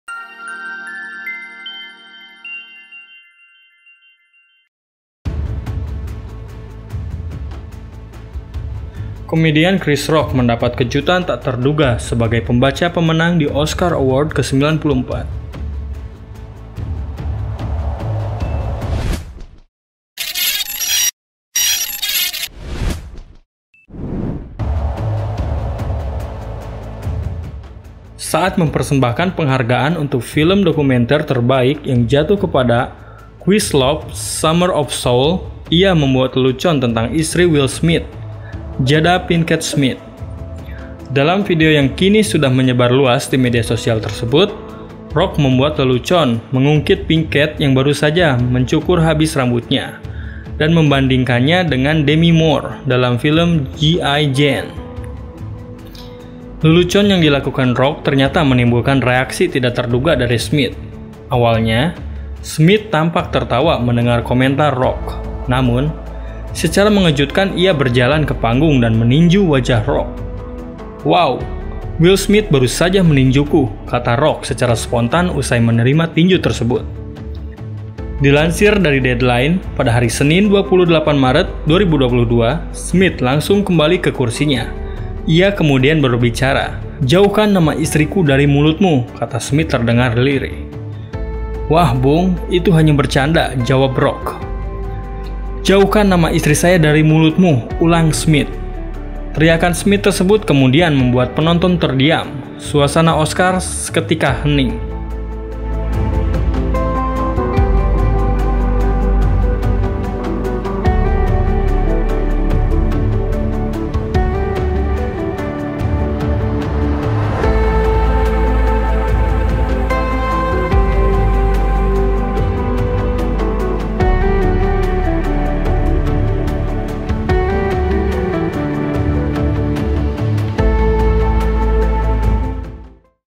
Alasan Will Smith Tinju Chris Rock di Oscar
Komedian Chris Rock mendapat kejutan tak terduga sebagai pembaca pemenang di Oscar Awards ke-94.

Saat mempersembahkan penghargaan untuk Film Fitur Dokumenter Terbaik yang jatuh ke Questlove's Summer of Soul, ia membuat lelucon tentang istri Will Smith, Jada Pinkett Smith.

Lelucon yang dilakukan Rock ternyata menimbulkan reaksi tidak terduga dari Smith.